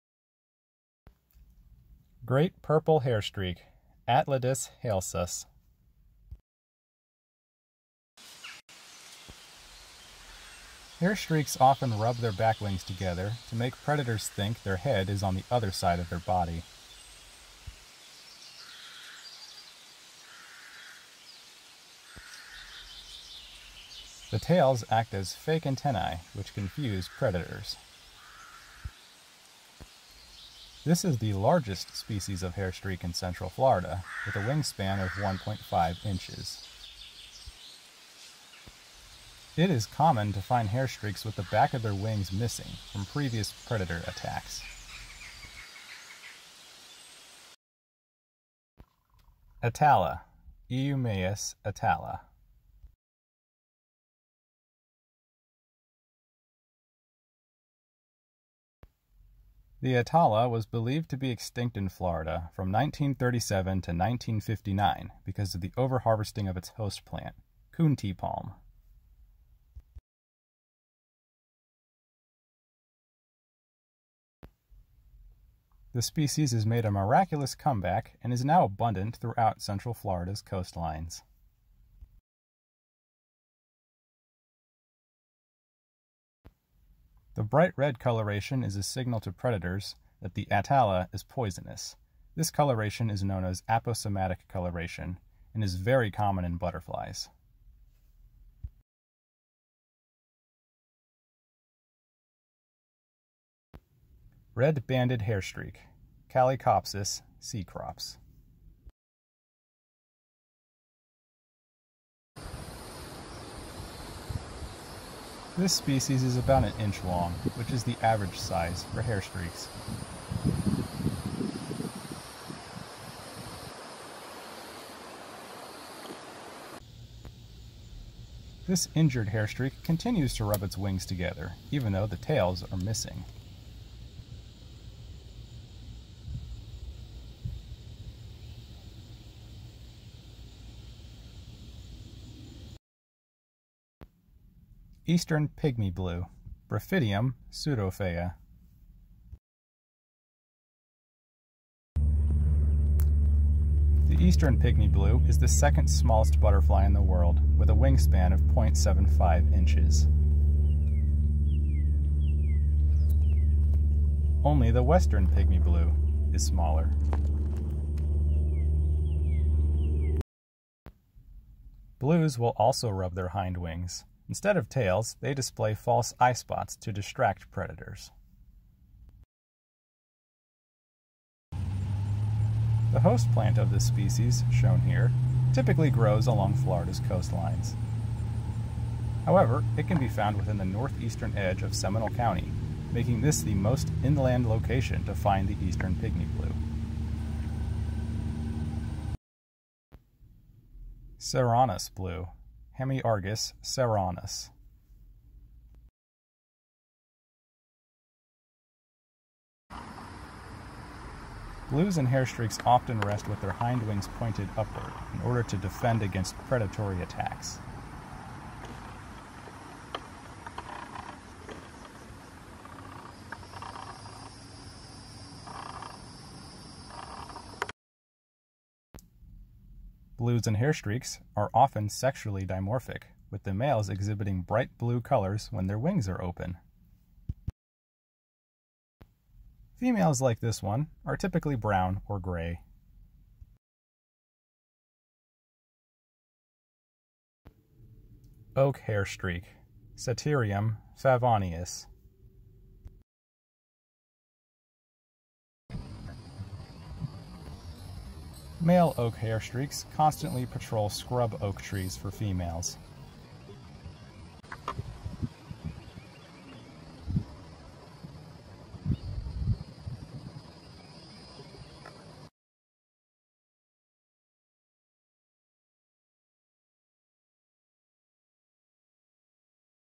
Hairstreaks and Blues of Central Florida! (Family Lycaenidae)
Note that this is NOT a comprehensive guide to all Hairstreaks and Blues of Central Florida. There are many more, however these are some of the most notable ones...and the only ones I could find in my county.